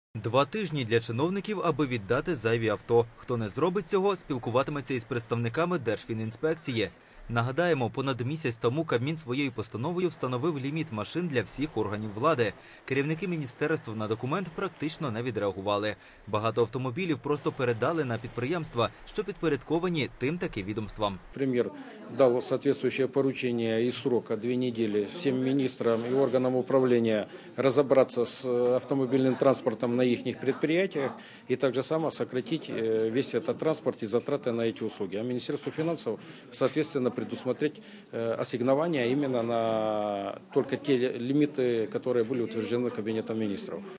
Урядовцям доведеться віддати автомобілі
ubr.ua - украинский бизнес ресурс : Два тижні для чиновників, аби віддати зайві авто. Хто не зробить цього - спілкуватиметься із представниками Держфінінспекції.
Нагадаємо, понад місяць тому Кабмін своєю постановою встановив ліміт машин для всіх органів влади. Керівники міністерств на документ практично не відреагували. Багато автомобілів просто передали на підприємства, що підпорядковані тим таки відомствам.
Дивіться також: Чиновники у метро або Кругообіг автомобілів у Кабміні&#65279;
Петро Андрєєв, голова Державної фінансової інспекції: &quot;Премьер дал соответствующее поручение и строка две недели министрам и органам управления разобраться с транспортом на их предприятиях и сократить транспортные затраты на ети услуги. А Минфину выдать ассигнования на лимиты, которые были утверждены Камином&quot;. .